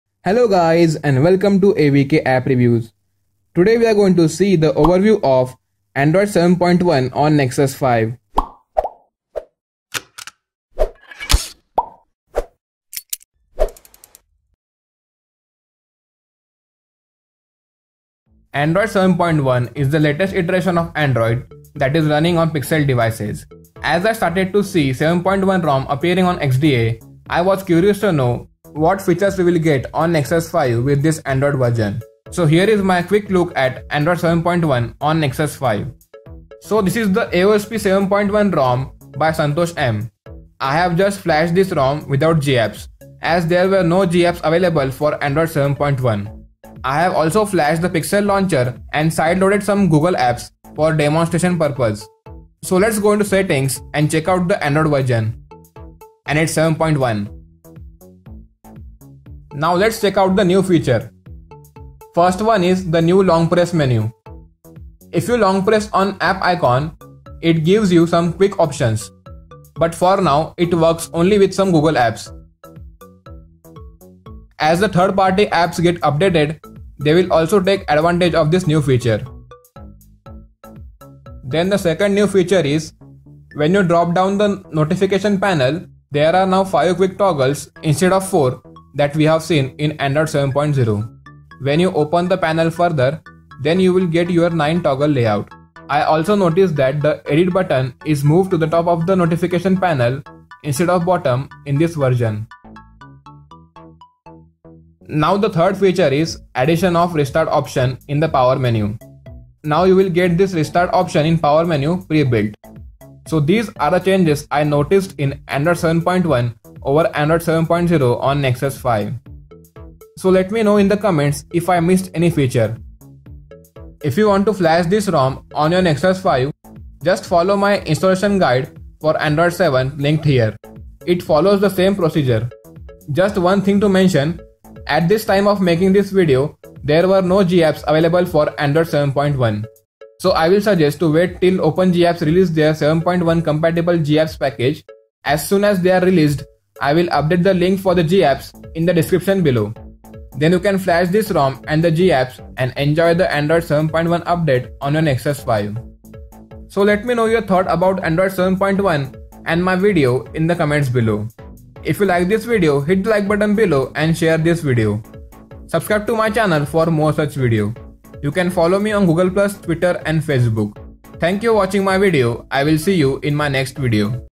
Android 7.1 On Nexus 5 !
This is the quick overview of Android 7.1 on Nexus 5. This video shows the new features of android 7.1 , which are available on nexus 5. So enjoy this latest iteration of Android on Nexus 5 !!!